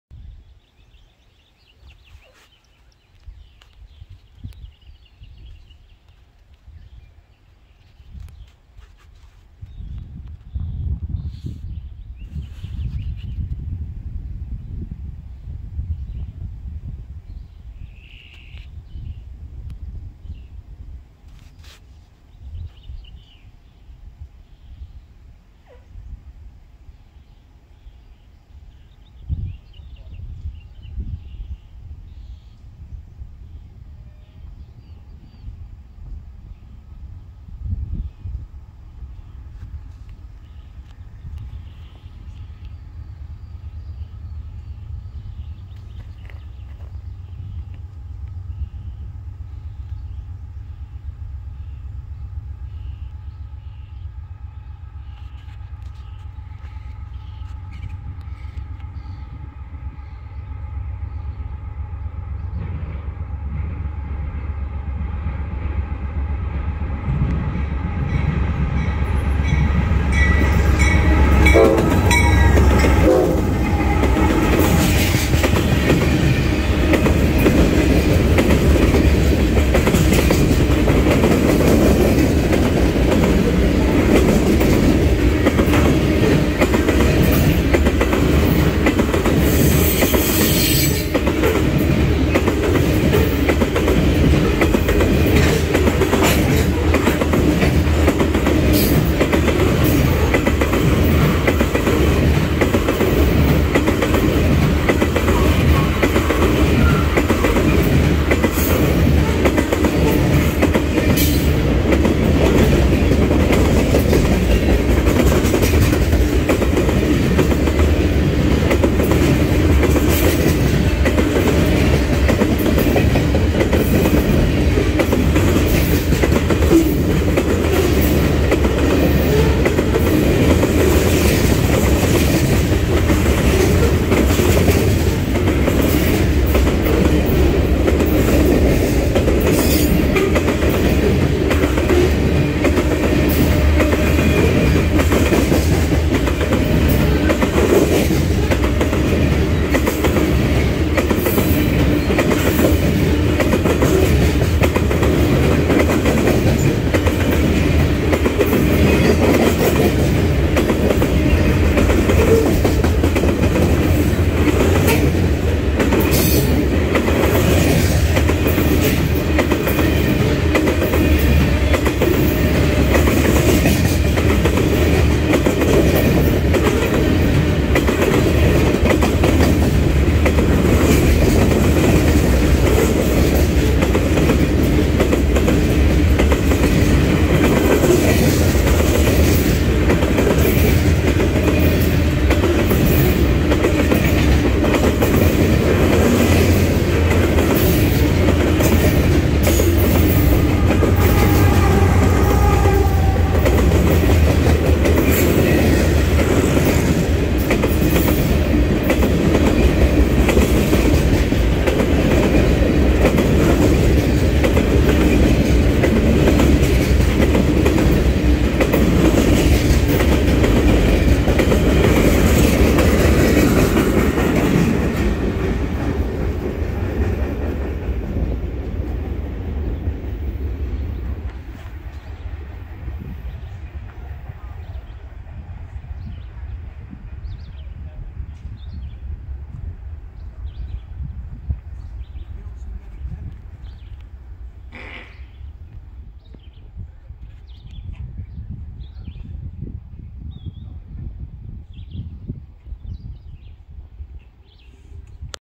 Norfolk Southern #4026 leads a 119 car manifest at Lewistown, PA (05/27/2023)
10G, after having cut a lot of cars off the back in Altoona